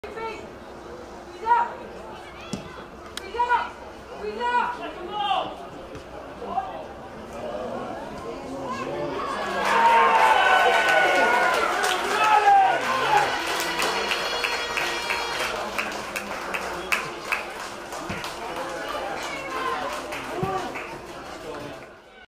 Durham 0 Yeovil Town 2
Sarah Wiltshire currently top goal score in FAWSL puts the game to bed for Yeovil Town Ladies with a cool finish.